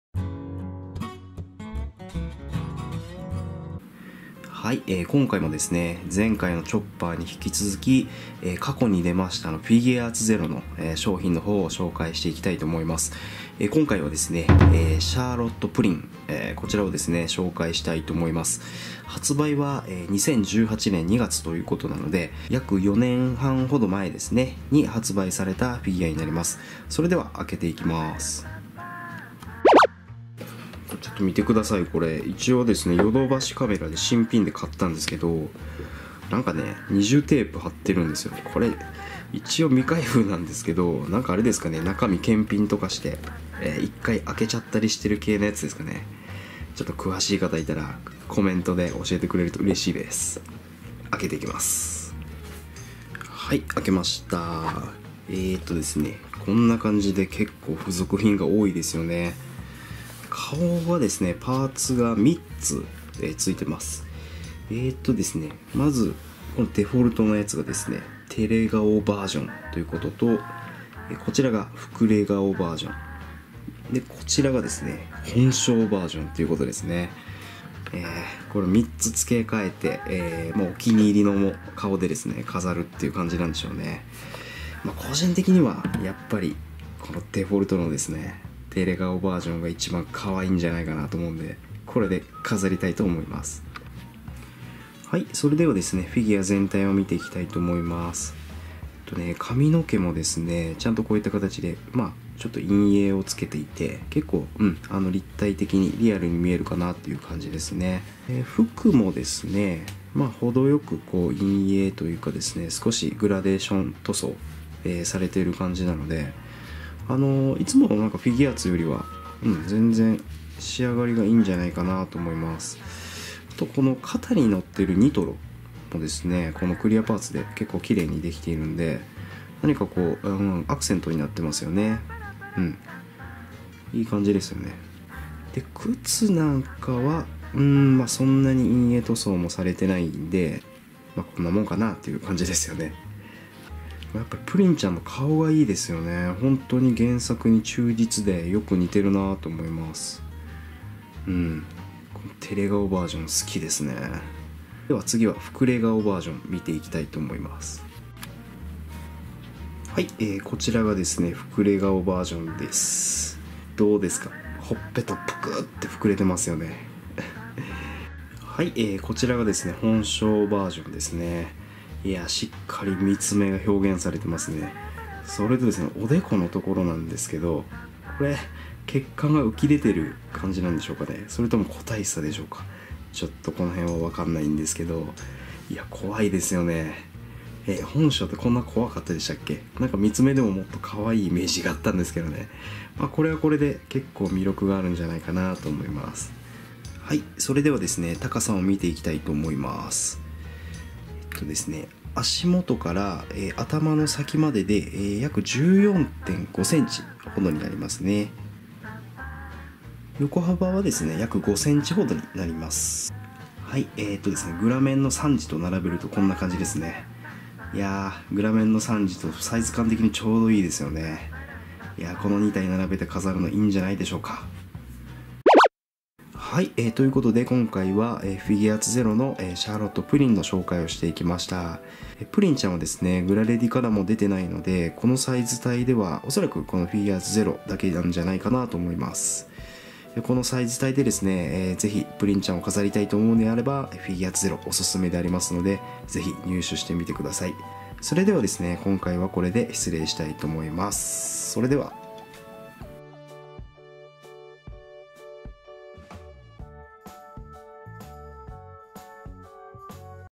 【フィギュア開封動画】Figuarts ZERO シャーロット・プリンの開封レビューです！■THE GRANDLINE seriesとマッチするフィギュア■
Figuarts ZERO シャーロット・プリンの開封レビューです！
発売は2018年2月ということなので、約4年半前に発売されたフィギュアの開封です。
３パターンの顔と腕の付け替えパーツ付きフィギュアです。
動画内では全てのパターンのパーツを紹介しています。

出来が良い上に、価格もお手頃（ヨドバシカメラ参考価格：3950円）の本当にオススメ出来るフィギュアです！
最近のフィギュアーツは本当に最高♪